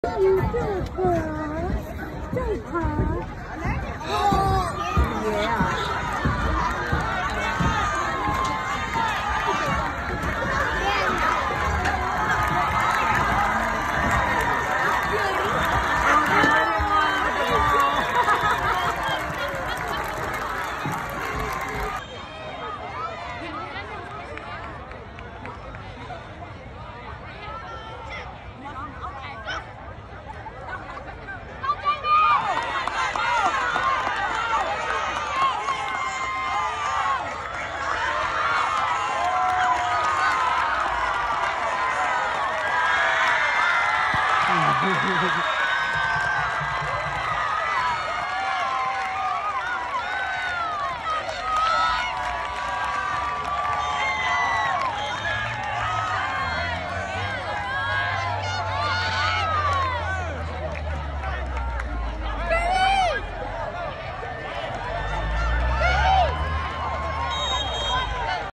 Henderson Middle School student with Down Syndrome scores touchdown
Tonight was a night Josh will never forget. He had the opportunity to suit up and make a special play during a football game against Spring Hill Junior High.